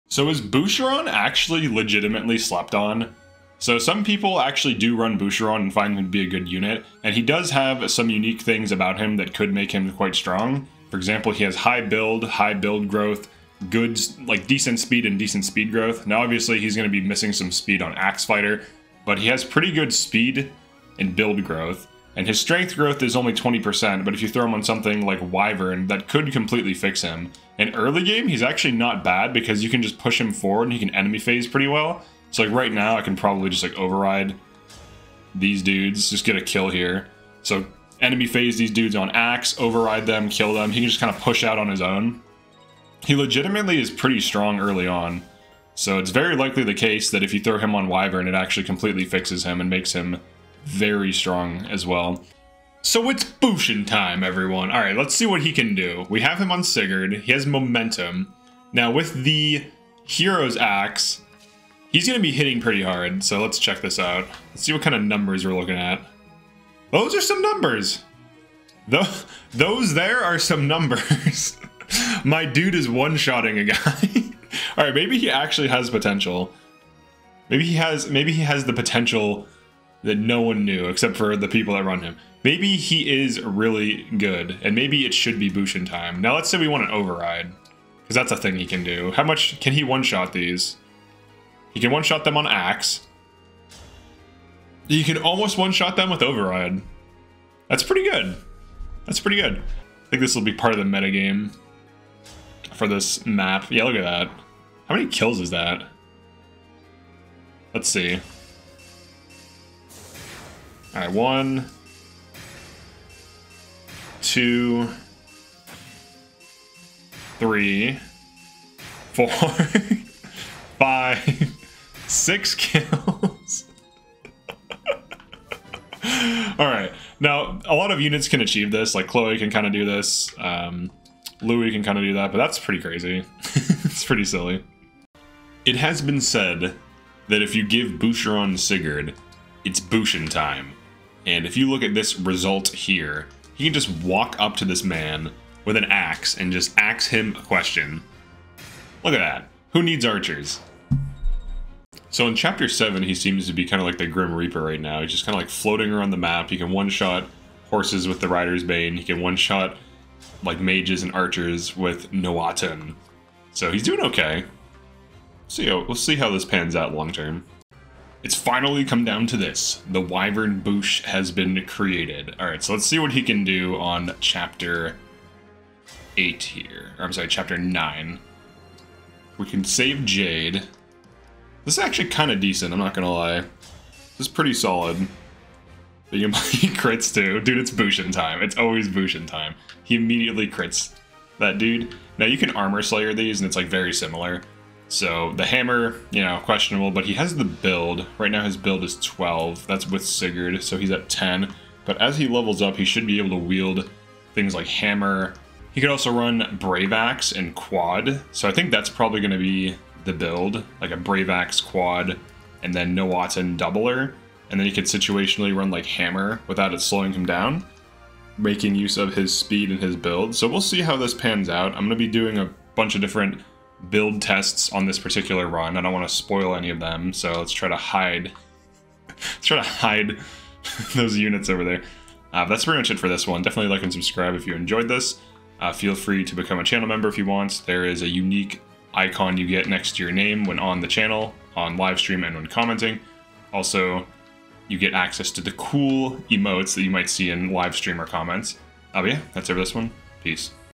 BOUCHERON is S TIER?
In this video I demo the potential of Boucheron on base game no DLC/stat boosters being used as one of my early game hard carries.

Boucheron has high BLD/SPD and can handle heavy axes easily but has lower STR/STR growth making him a unique build archetype.